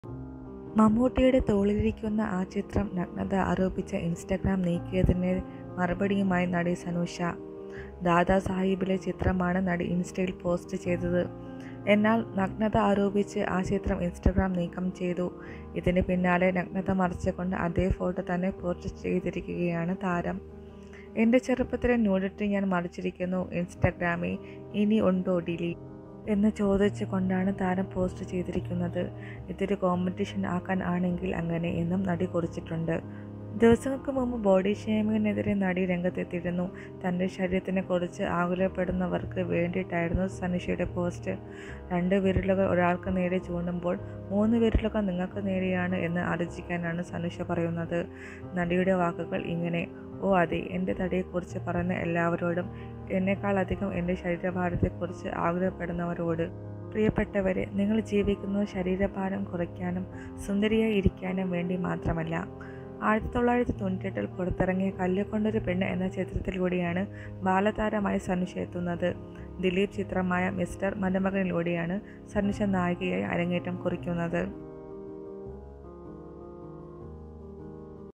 മമ്മൂട്ടിയുടെ തോളില്‍ ഇരിക്കുന്ന ചിത്രം ഇന്‍സ്റ്റഗ്രാം നീക്കി നഗ്നത മറച്ച് വീണ്ടും പോസ്റ്റ് ചെയ്തു
STAR STARZZ